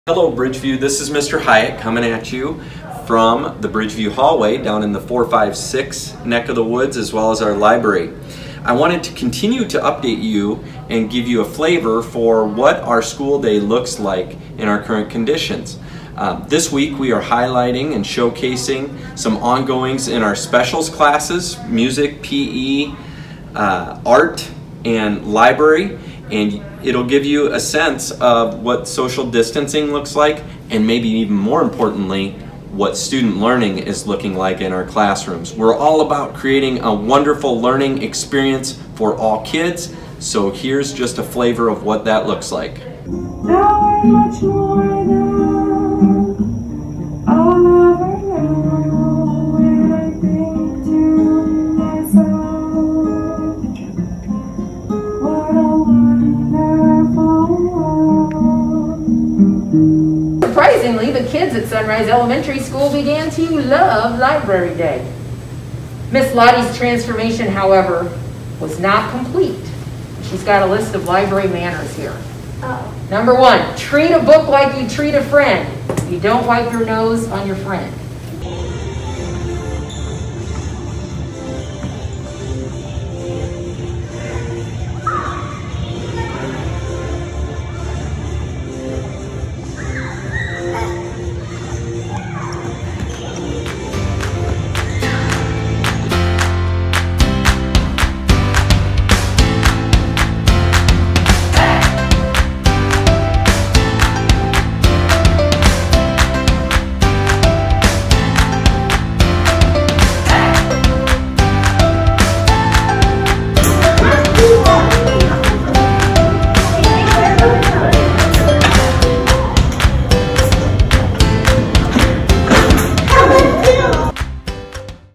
BV Specials
Mr. Hiatt shows you what it's like in specials classes at Bridgeview Elementary in LeClaire, IA.